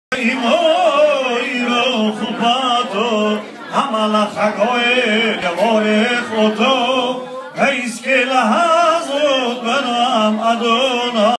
007.MPG, Misha Normatov,
MAZAL TOV - BRITH MILA OF GOSHUA-MICKAEL YUSUPOV BEN ALISA & EMANUEL YUSUPOV, HASANDAK RAFIK YUSUPOV, HASANDAK RISHON SAFAN AMINOV (GREAT GRAND FATHER)
Rn "X.O. DAMIKEL" QUEENS , NY 07/16/2014